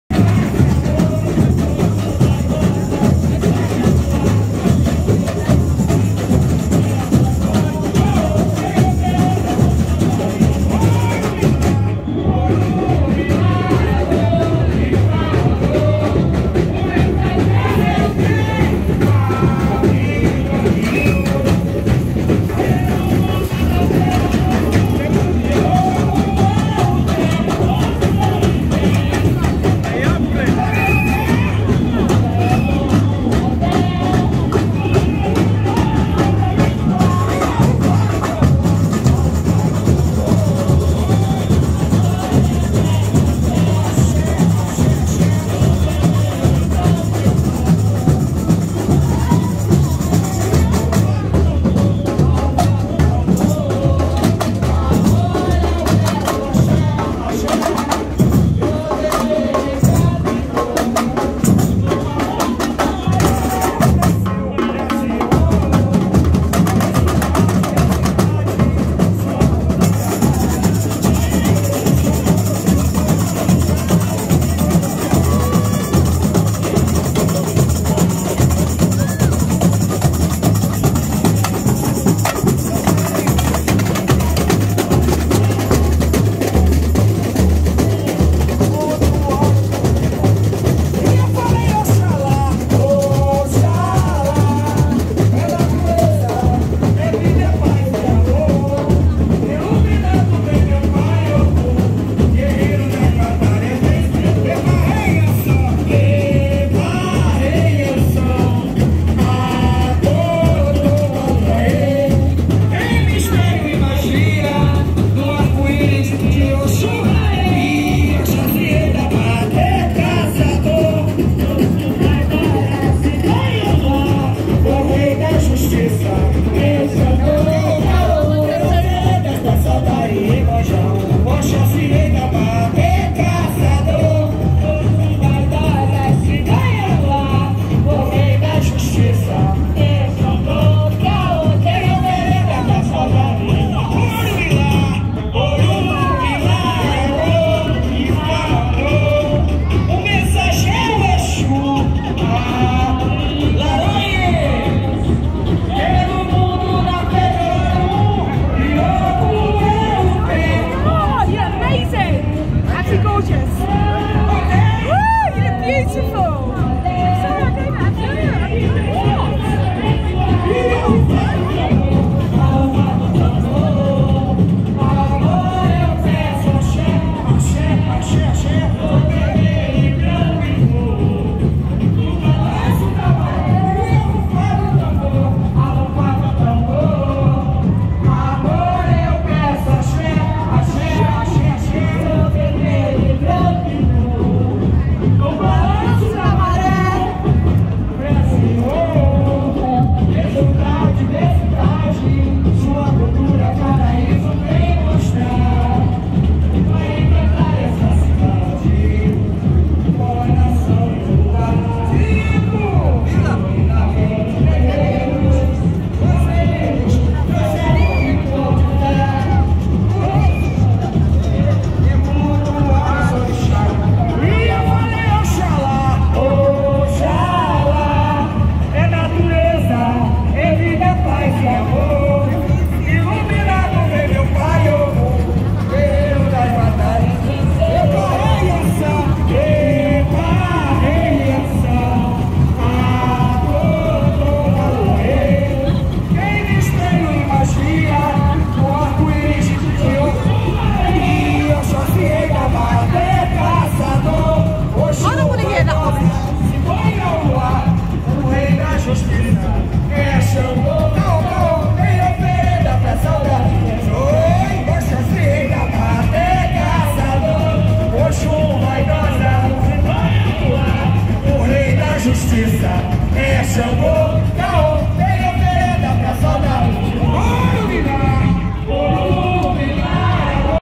🇬🇧 NOTTING HILL CARNIVAL 2022, MONDAY 29th of AUGUST 2022, BIG CROWD AT THE CARNIVAL, STUNNING, 4K
🇬🇧 NOTTING HILL CARNIVAL 2023 WILL BE AMAZING!  WILL YOU BE THERE?